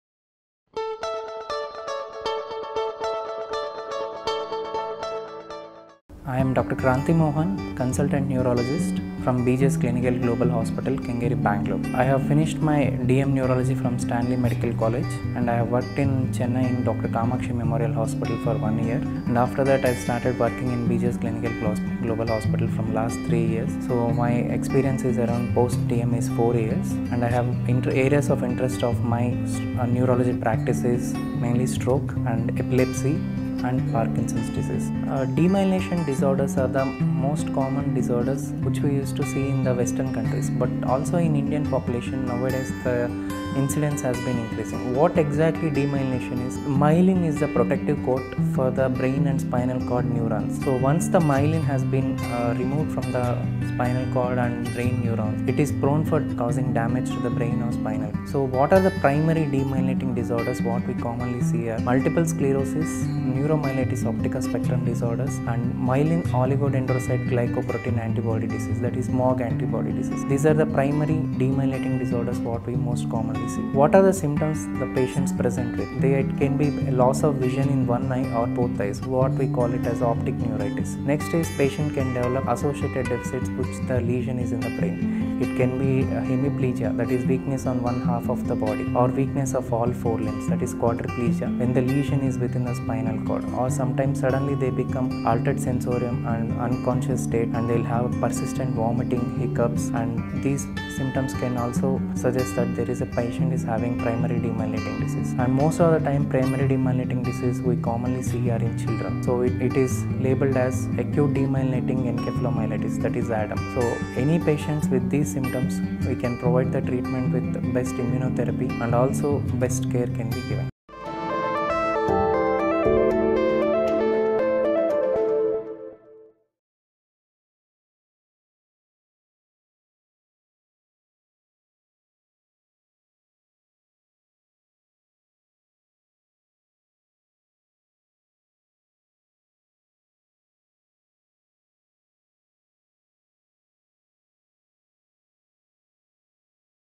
Demyelination Disorder | Dr. Kranthi Mohan
Myelin is a protective covering around the body section of the nerve cells in our central and peripheral nervous system. It protects, strengthens, and insulates our nerve cells. A demyelinating disease is a medical condition in which the myelin in our brain, spinal cord, and nerves gets damaged.

If you suffer from a demyelinating disease, your immune system will attack the cells that make up your myelin. This causes changes in the way your nerves communicate and function. Common demyelinating diseases affecting the CNS include -

-Multiple sclerosis (MS)
-Neuromyelitis optica spectrum disorder (NMOSD)
-Acute disseminated encephalomyelitis (ADEM)
-Central pontine myelinolysis (osmotic demyelination syndrome)
-Transverse myelitis (TM)
-Progressive multifocal leukoencephalopathy (PML)

Treatment for a demyelinating disease varies according to the condition you have. Your neurologist will work closely with you to manage symptoms. Treatment could include:

Medications: Disease-modifying therapies (DMT) are used together with symptomatic treatment. The goal is to minimize the attack's effects, modify the disease's course, and manage the symptoms.
Physical and occupational therapy: Physical and occupational therapies help increase muscle strength and resolve problems with coordination and balance.